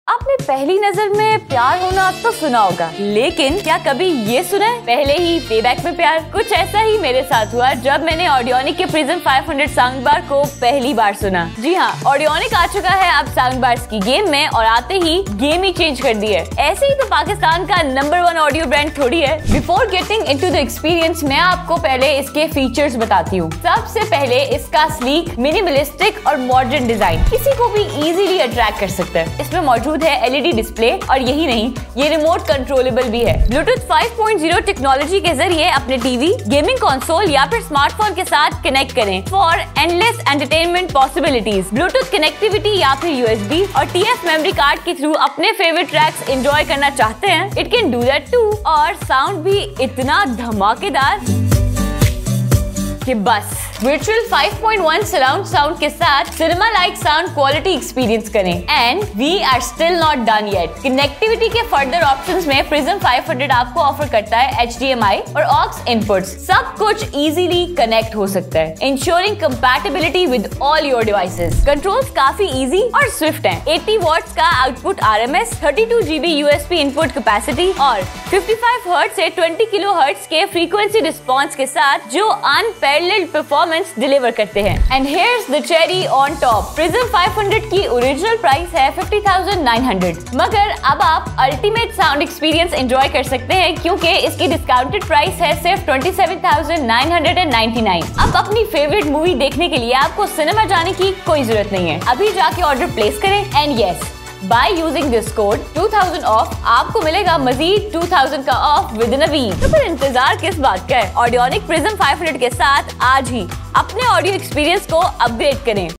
Prism 500 Sound Bar by Audionic
Enhance your audio experience with the Prism 500 Sound Bar by Audionic. Get clear sound and deep bass for better entertainment. Easy to connect and looks great in your home.